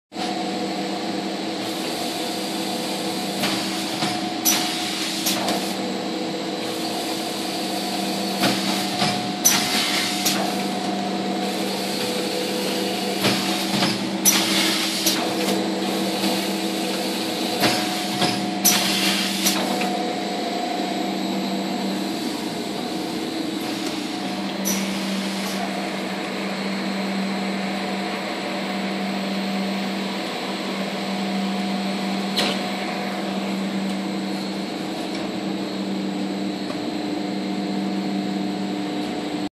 Testing model TE-25
Adjusted for packaging medical sheets rolls